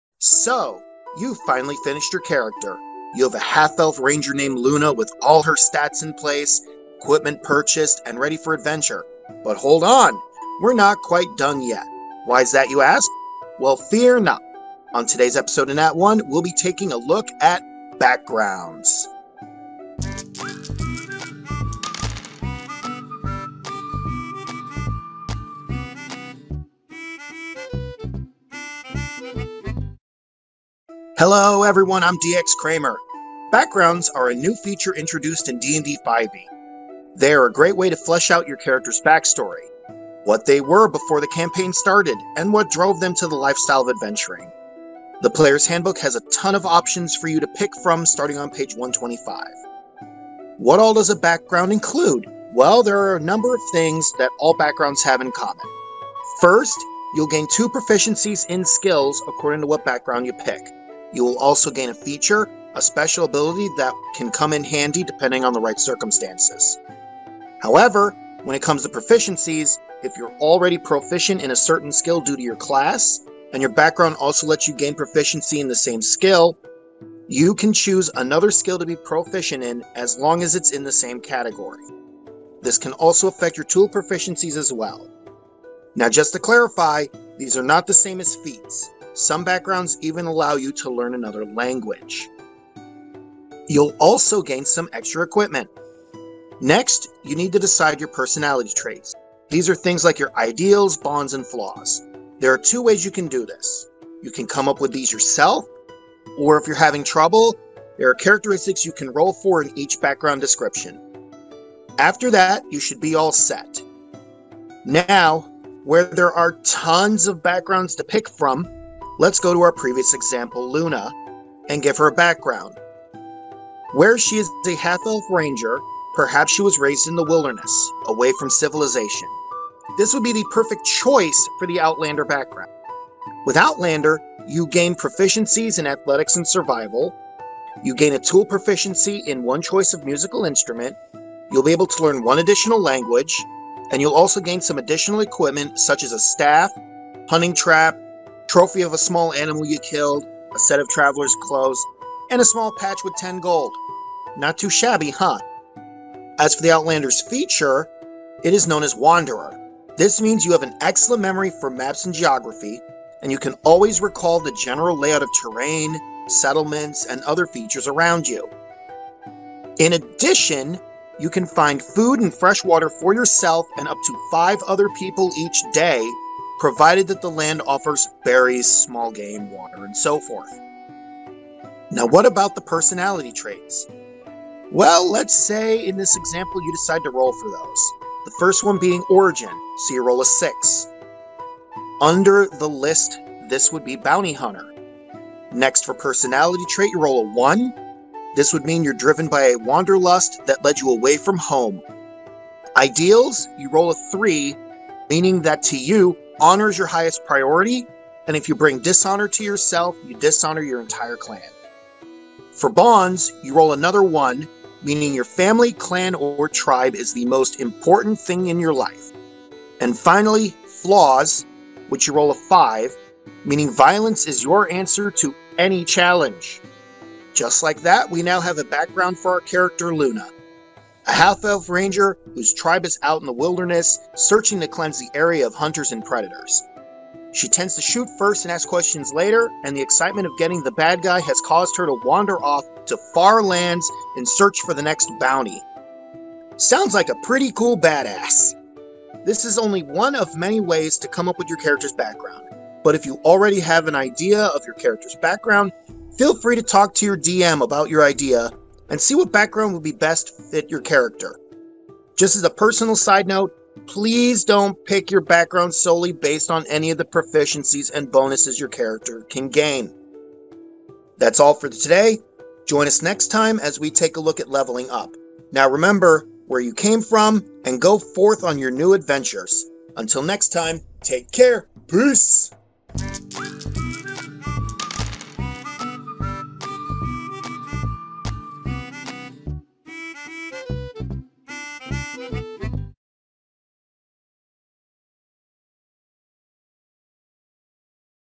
Nat 1: Episode 18 - Backgrounds (Remastered)
Today we talk about how to create your character's background in D&D 5e.